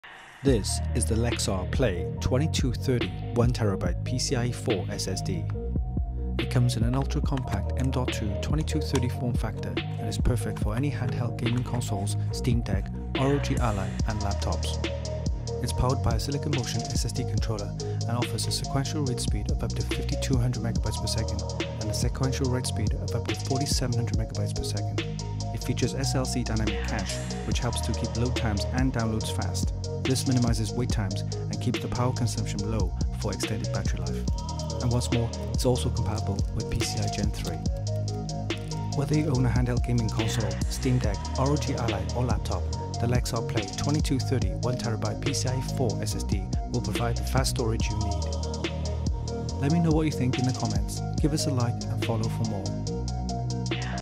The perfect SSD for your Steam Deck, ROG Ally or Laptop 👍 the Lexar PLAY 2230 🔥@lexarmemory
You can buy the Lexar PLAY 2230 1TB PCIe 4.0 SSD for around USD $109.99 $85 (sale) from Amazon